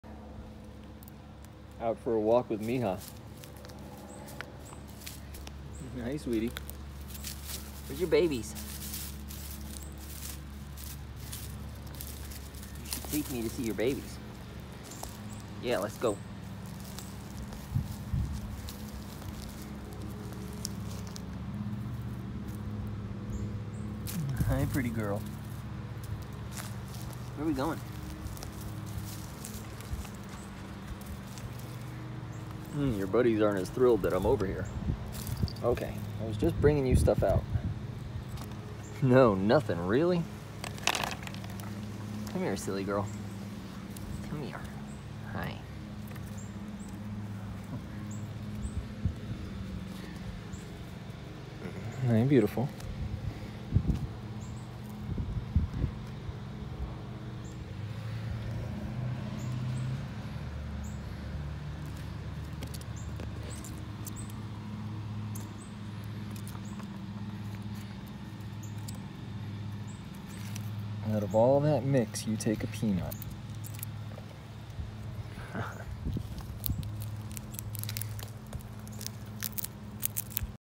Visiting Mija
Mija is one of our releases who just had kits in the wild.